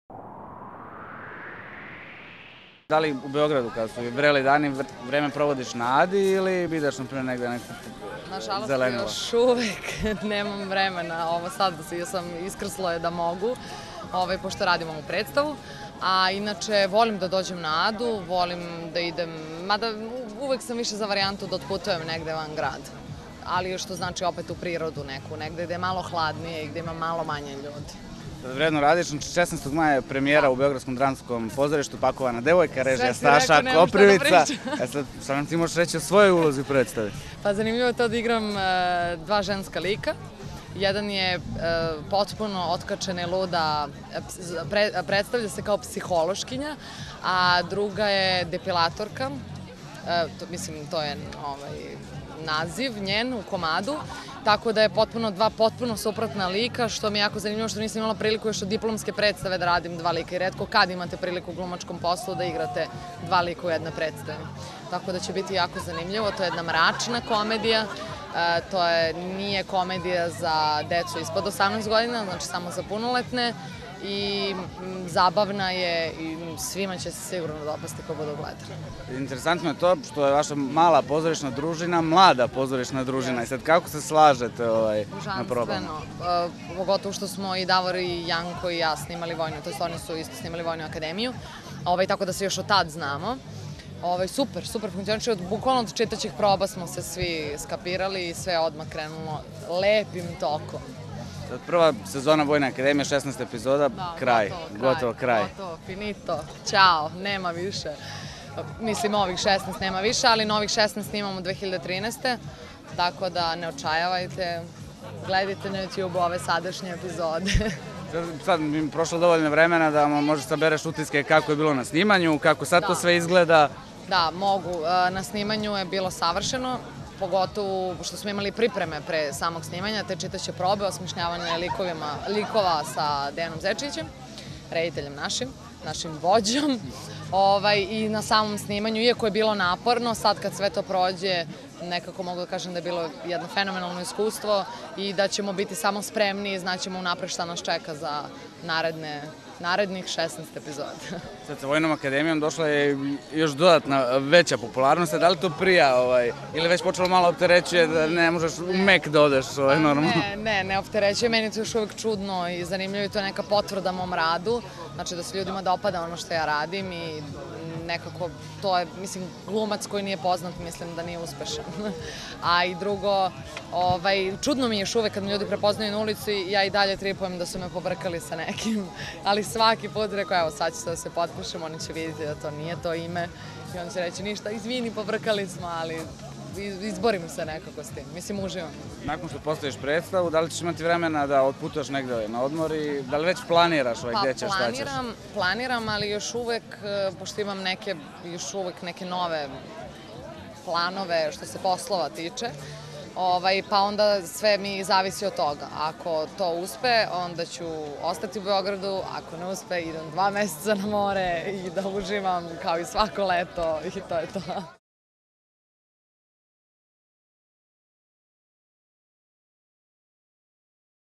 Jelisaveta Orašanin: Posle vojske vreme je za mračnu komediju
Mlada glumica Jelisaveta Orašanin govori o snimanju serije Vojna akademija, predstavi Pakovana devojka, ali i leto koje će ukoliko ne bude radila provesti sa svojim dečkom i kolegom Vukom Kostićem.